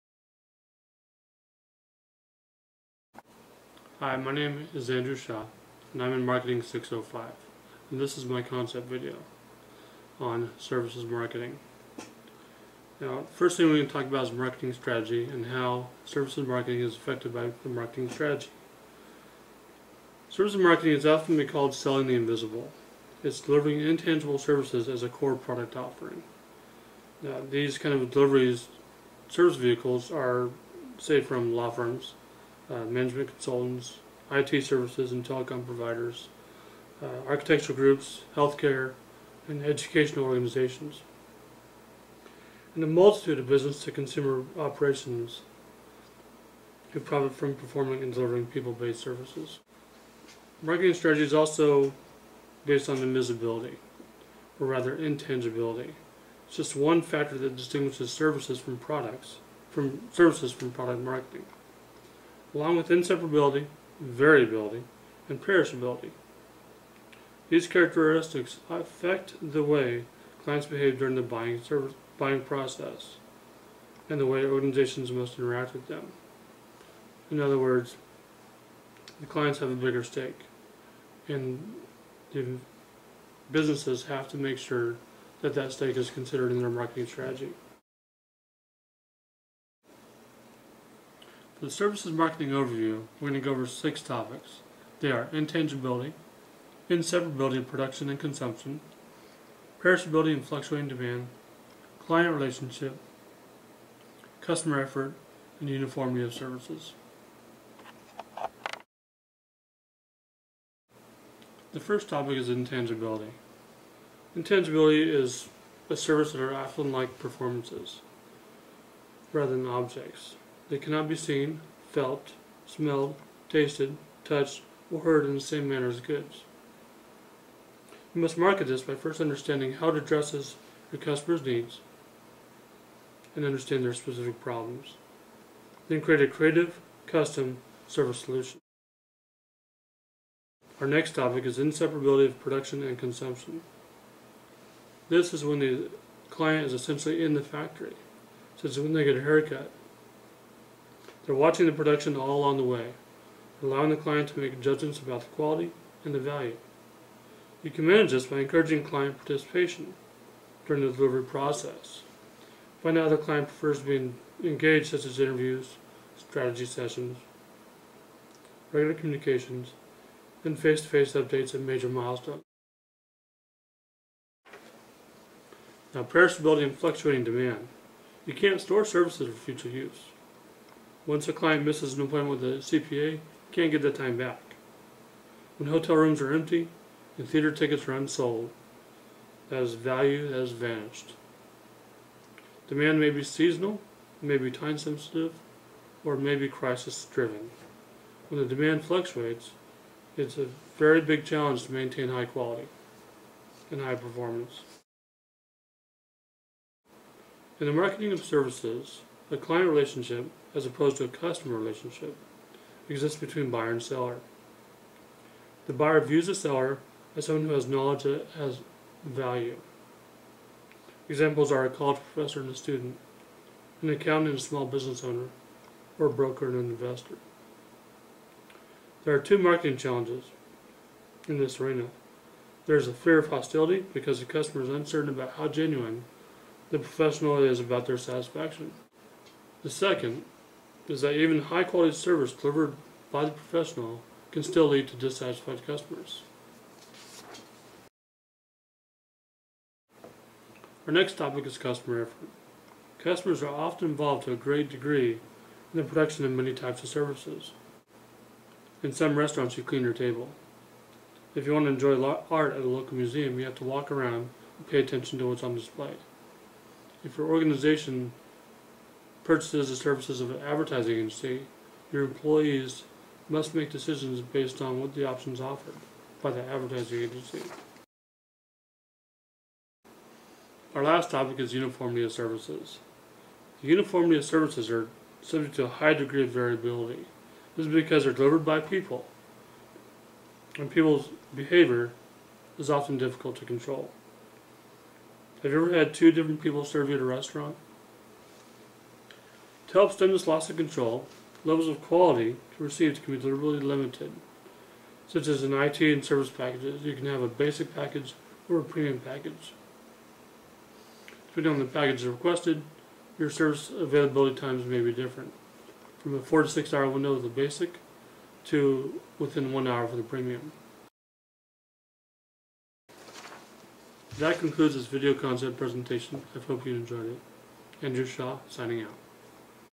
MKTG 605 Video Concept
Services Marketing Overview
• Intangibility
• Inseparability of Production and Consumption
• Perishability and Fluctuating Demand
• Client Relationship
• Customer Effort
• Uniformity of Services